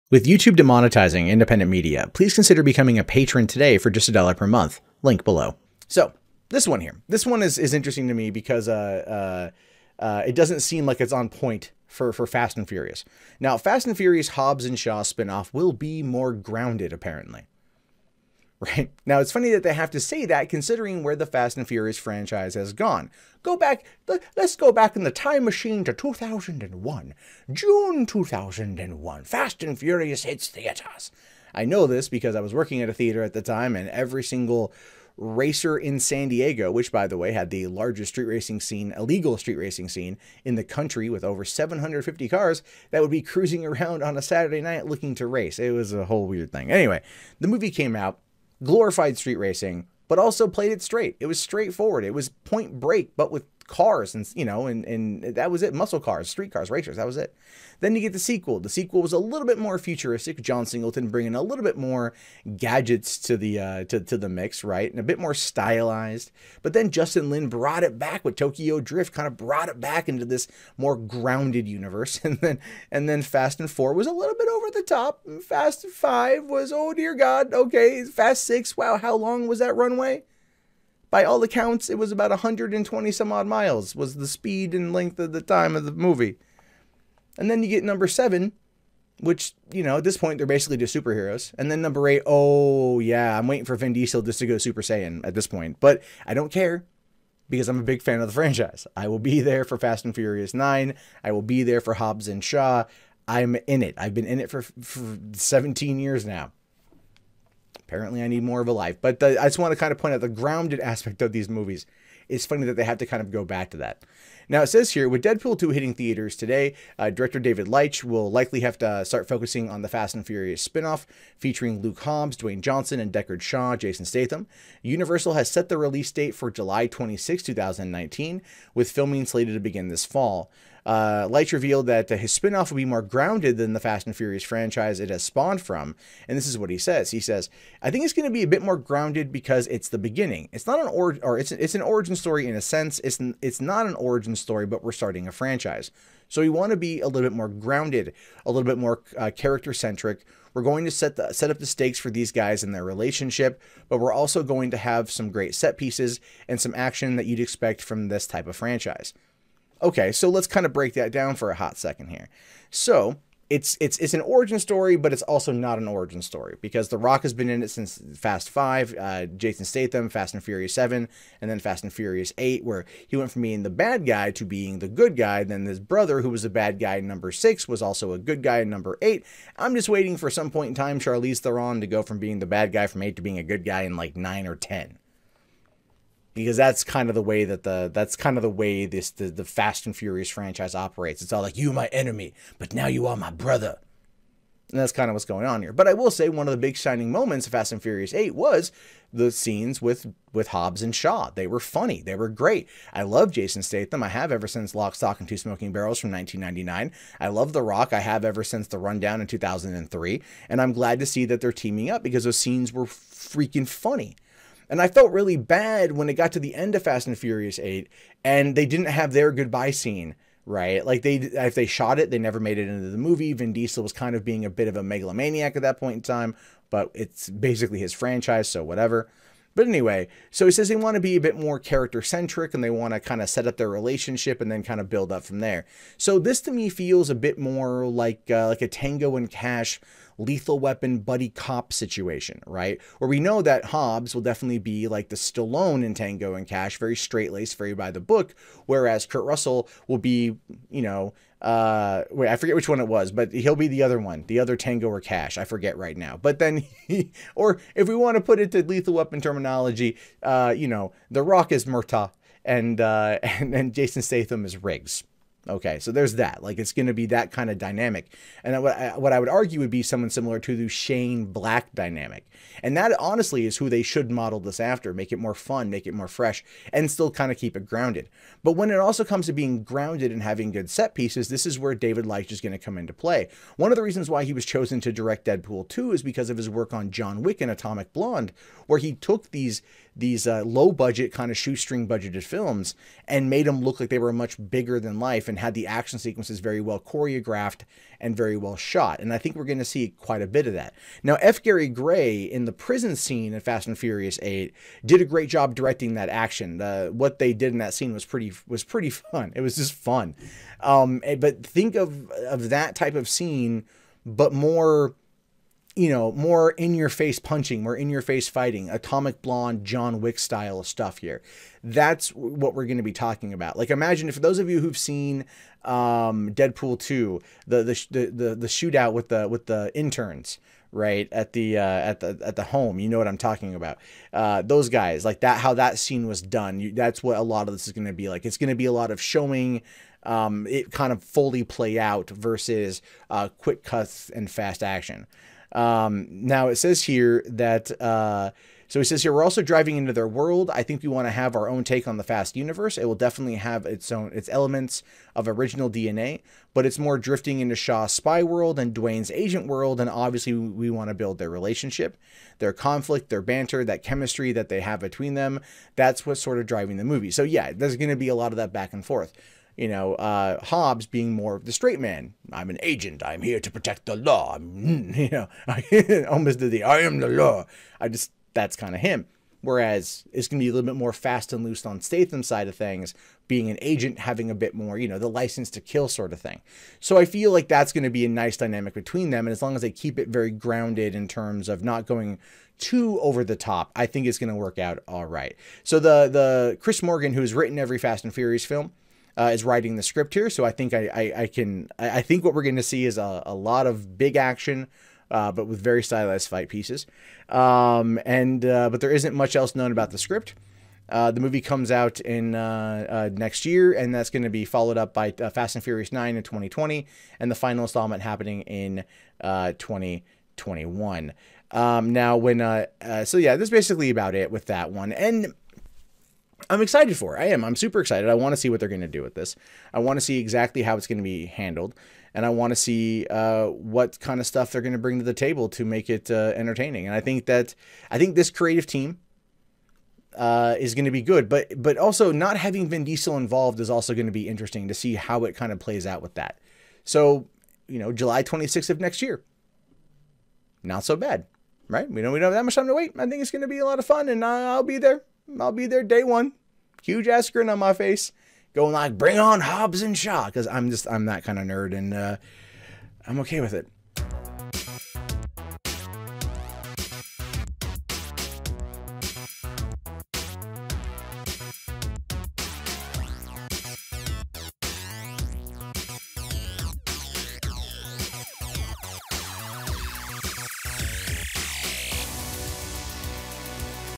HOBBS & SHAW spin-off to be more "Grounded"
At this point the FAST & FURIOUS movies are basically superhero films centered around Family. Loyatly, and fast cars. And that might be changing in David Leitch's HOBBS & SHAW spin-off. Leitch wants to make the film starring Dwayne Johnson, and Jason Statham more grounded...and he's just the guy to do it considering his work with JOHN WICK, ATOMIC BLONDE, and now DEADPOOL 2.